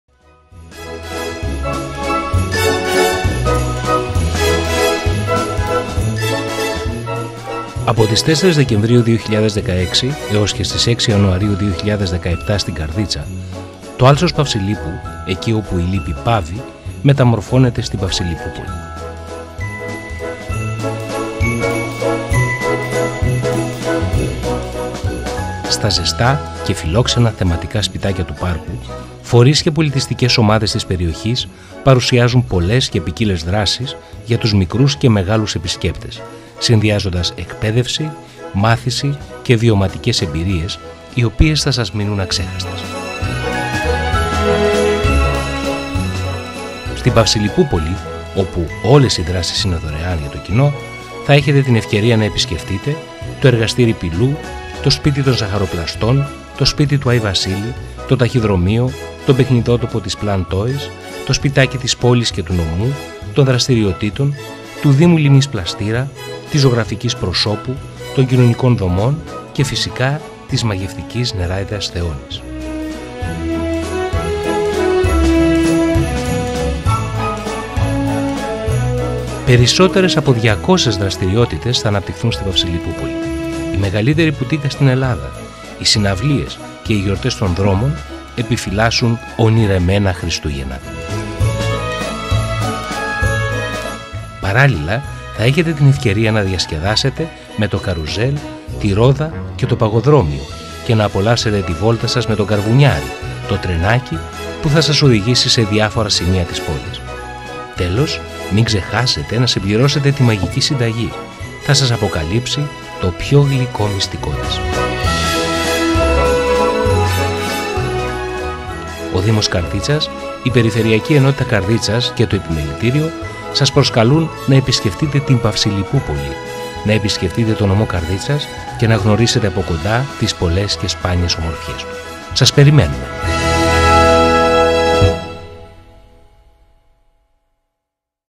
Παυσιλυπούπολη 2016-2017
Με φόντο τις κορυφές των Αγράφων, τη μαγευτική λίμνη Πλαστήρα, στην όμορφη και φιλόξενη πόλη του ποδηλάτου, στην πόλη της Καρδίτσας, υλοποιείται το Χριστουγεννιάτικο θεματικό Πάρκο, με τίτλο, «Παυσιλυπούπολη».

Από τις 4 Δεκεμβρίου 2016 ως τις 6 Ιανουαρίου 2017, το άλσος Παυσιλύπου – εκεί όπου η λύπη παύει- μεταμορφώνεται στην Παυσιλυπούπολη.

Στα ζεστά και φιλόξενα θεματικά σπιτάκια του πάρκου, φορείς και πολιτιστικές ομάδες της περιοχής παρουσιάζουν πολλές και ποικίλες δράσεις για τους μικρούς και μεγάλους επισκέπτες, συνδυάζοντας εκπαίδευση, μάθηση και βιωματικές εμπειρίες οι οποίες θα σας μείνουν αξέχαστες.

Στην «Παυσιλυπούπολη» όπου όλες οι δράσεις είναι δωρεάν για το κοινό, θα έχετε την ευκαιρία να επισκεφθείτε: το εργαστήρι πηλού, το σπίτι των ζαχαροπλαστών, το σπίτι του ΑΪ Βασίλη, το ταχυδρομείο, τον παιχνιδότοπο της Plan Toys, το σπιτάκι της πόλης και του Νομού, των δραστηριοτήτων, του Δήμου Λίμνης Πλαστήρα, της ζωγραφικής προσώπου, των κοινωνικών δομών και φυσικά της μαγευτικής νεράϊδας του νερού, της νεράϊδας ΘΕΟΝΗ. Περισσότερες από 200 δραστηριότητες θα αναπτυχθούν στην Παυσιλυπούπολη, οι οποίες σε συνδυασμό με τις συναυλίες και τις γιορτές των δρόμων, επιφυλάσσουν ονειρεμένα Χριστούγεννα.

Παράλληλα θα έχετε την ευκαιρία να διασκεδάσετε με το καρουζέλ και το παγοδρόμιο και να απολαύσετε τη βόλτα σας με τον «καρβουνιάρη», το τρενάκι που θα σας οδηγήσει σε διάφορα σημεία της πόλης.

Τέλος, μην ξεχάσετε να συμπληρώσετε τη μαγική συνταγή. Θα σας αποκαλύψει το πιο γλυκό μυστικό της…

Ο Δήμος Καρδίτσας, η Περιφερειακή Ενότητα Καρδίτσας και το Επιμελητήριο Καρδίτσας σας προσκαλούν να επισκεφθείτε την «Παυσιλυπούπολη», να επισκεφτείτε το Νομό Καρδίτσας και να γνωρίσετε από κοντά τις πολλές και σπάνιες ομορφιές του…

Σας περιμένουμε